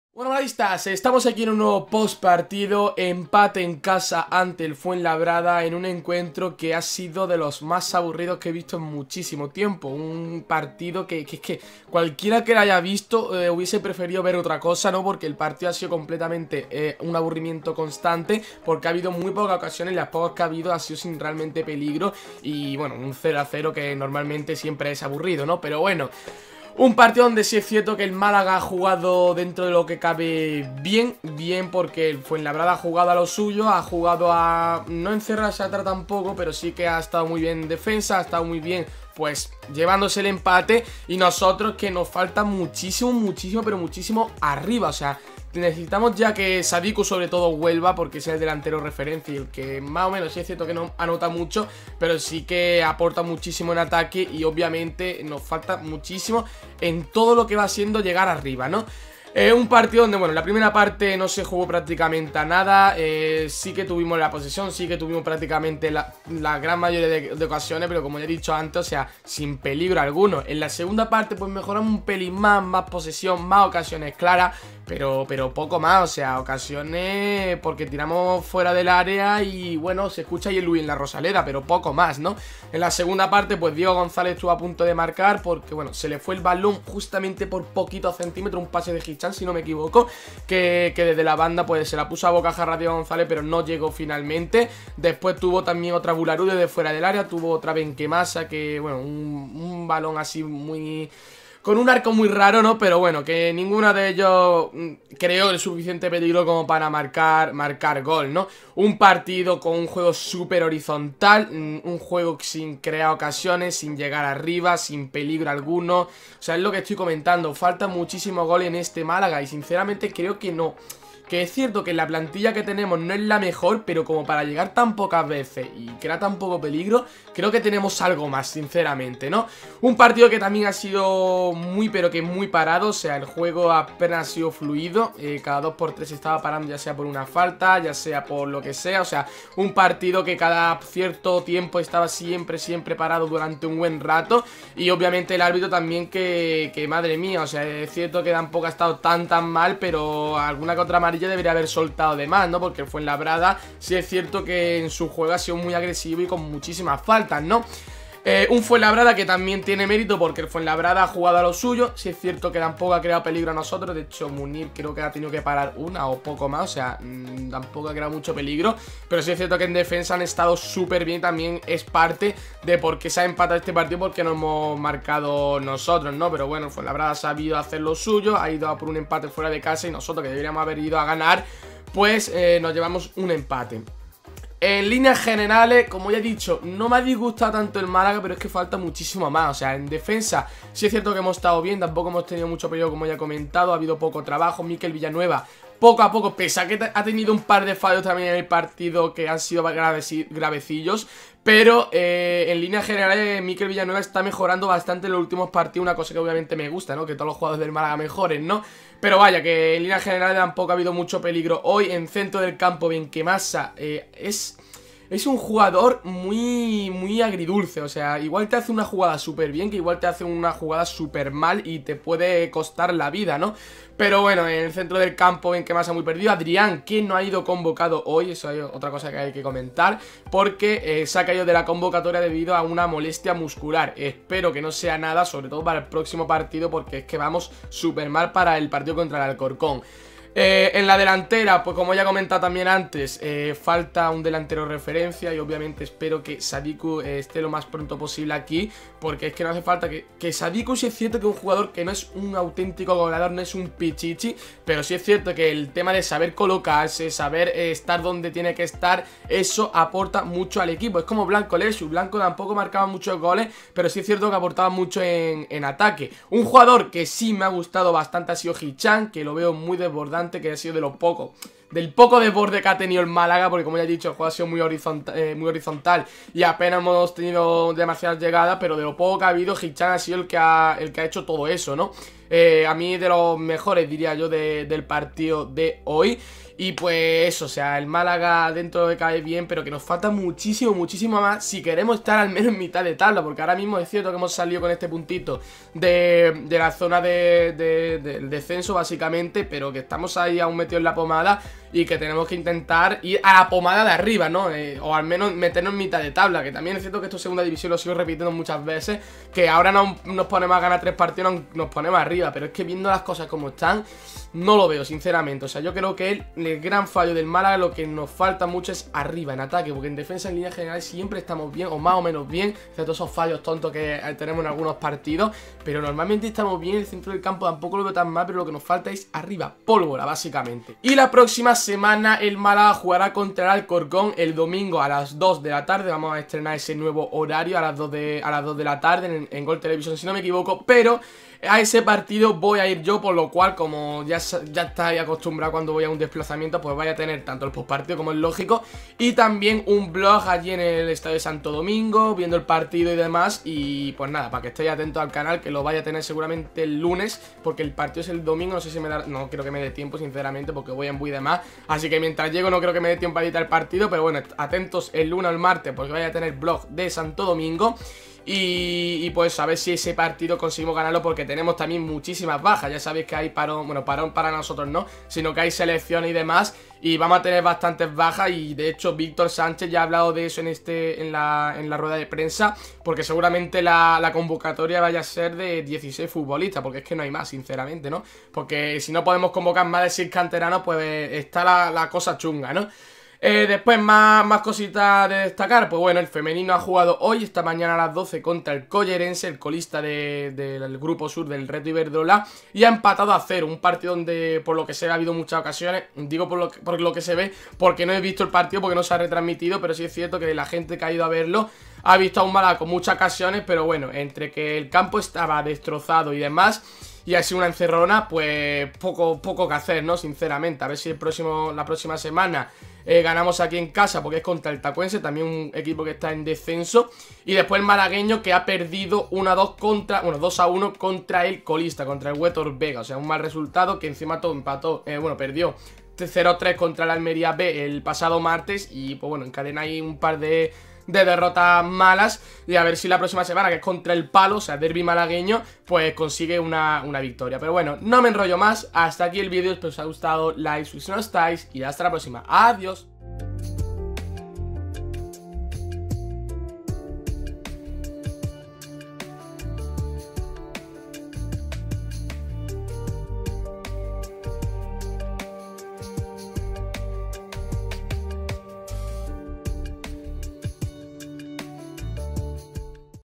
Málaga CF 0-0 CF Fuenlabrada | "ABURRIMIENTO" | J.15 LaLiga SmartBank 2019/20
Empate sin goles en La Rosaleda ante el Fuenlabrada.